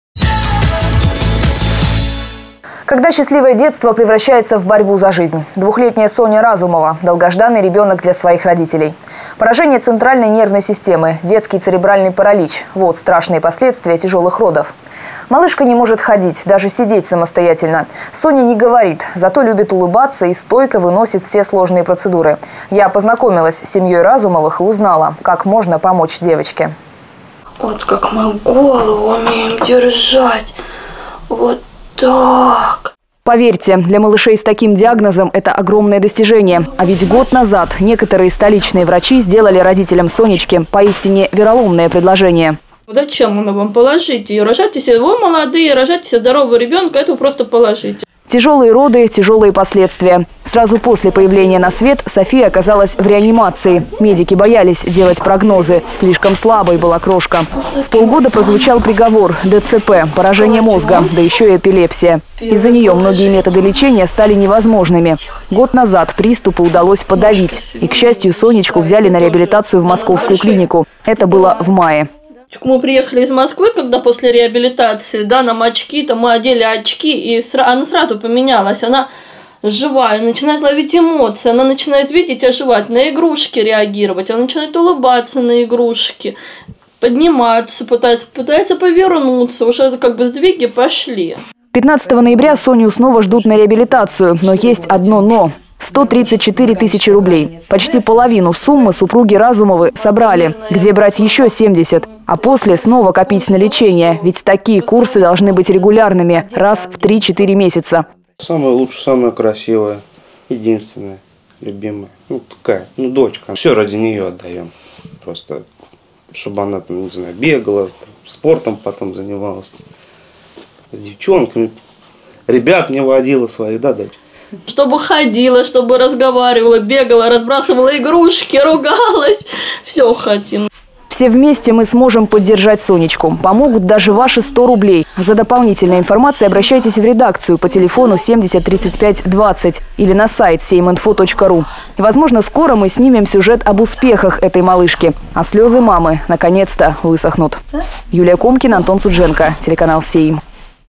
Двухлетняя Соня нуждается в помощи неравнодушных курян!
Когда счастливое детство превращается в борьбу за жизнь. Двухлетняя Соня Разумова - долгожданный ребенок для своих родителей. Поражение центральной нервной системы, детский церебральный паралич - вот страшные последствия тяжелых родов. Малышка не может ходить, даже сидеть самостоятельно. Соня не говорит, зато любит улыбаться и стойко выносит все сложные процедуры.